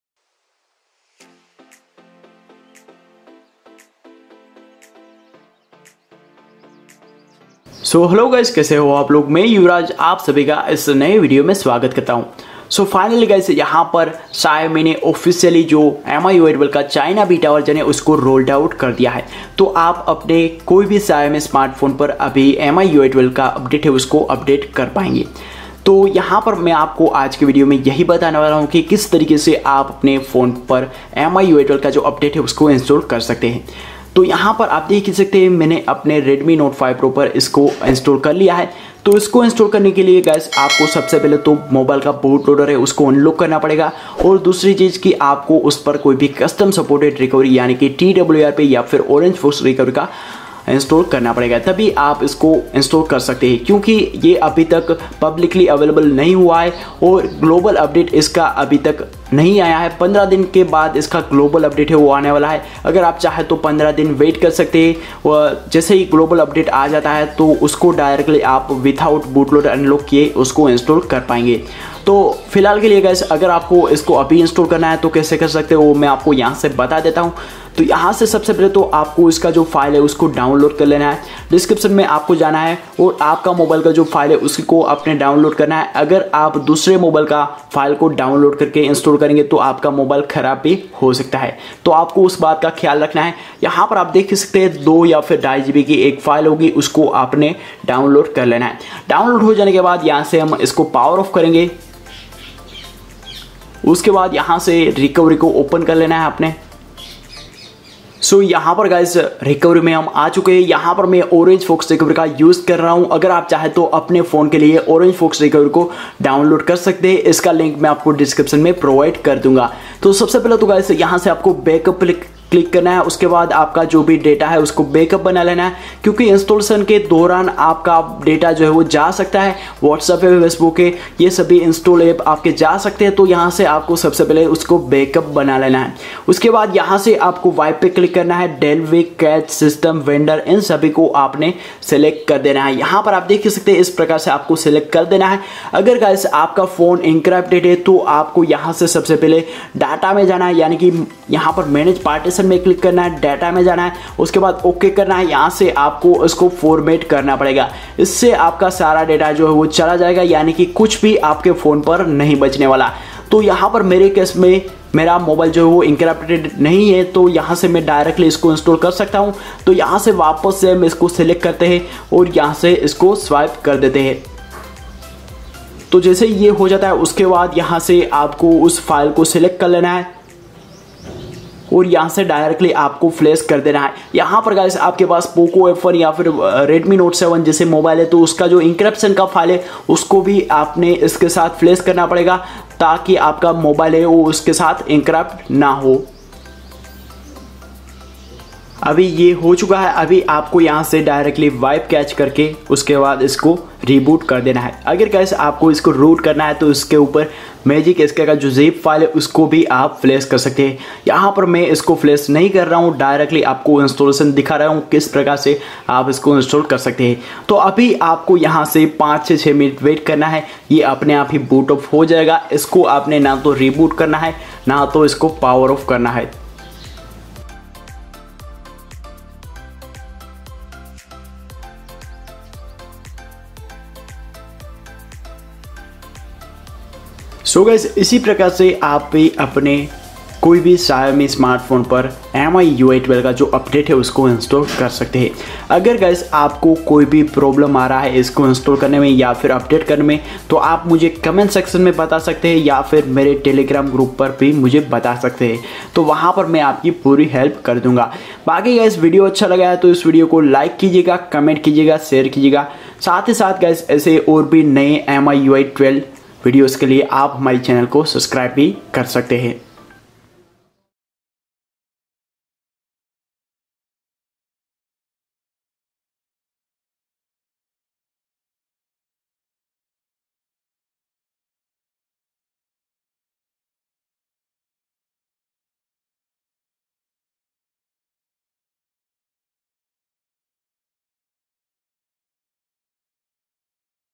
How To Install MIUI 12 Official Update On Any Xiaomi Phone
How To Install MIUI 12 Official Update On Any Xiaomi Phone - Get MIUI 12 Update On Any Xiaomi Phone - Hey, guys, I hope you like this video if you like den please hit the like button and don't forget to subscribe for more best miui themes.

(You can download all miui 12 updates using this app)

How to install miui 12 update

First, take the full backup of your data
Download correct file for your phone
Reboot to recovery
Wipe data/system/vendor/cache/delvick (If you're device encrypted then you have to formate date first)
Now install the update file
Install Magisk (Optional)
If you have redmi note 7,poco f1 or any other device that supports encryption then install encryption disabler
Reboot!

➞ Telegram Channel (For best miui content like ROMs, patches, guide,miui themes and more)